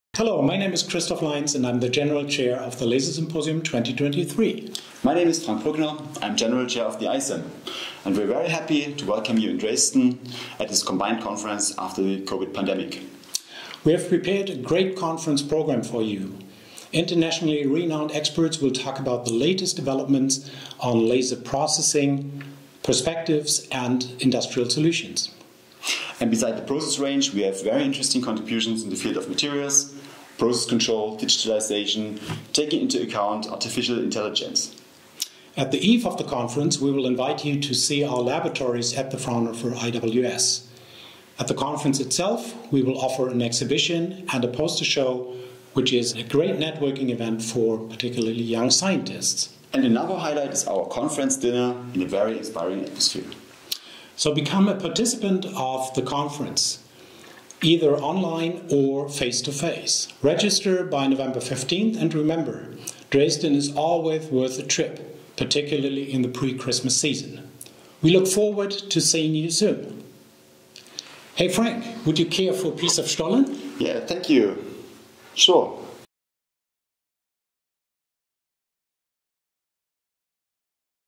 Join Laser Symposium and International Symposium Additive Manufacturing (ISAM) 2023
For the twelfth time in a row, Laser Symposium 2023 brings together world's leading laser experts, end-users and newcomers from both industry and academia. With a focus on latest laser developments and their suitability for industrial applications, it offers an ideal platform for scientific and technical exchange on various aspects of laser materials processing.

Additive manufacturing, also known as 3D printing, is becoming increasingly important for industrial applications. Fascinating engineering solutions paired with exciting economic benefits are strong drivers for R&D efforts worldwide. The International Symposium Additive Manufacturing (ISAM) highlights latest developments and challenges in additive manufacturing.

Combined into one joint conference, Laser Symposium and ISAM will provide a unique platform for the exchange of ideas and for industrial networking. As participant, you will benefit from overlaps in content and gain exciting insights into new topics.

Whether you prefer to attend in person or virtually, the conference offers the perfect setting for gaining new insights and exciting discussions.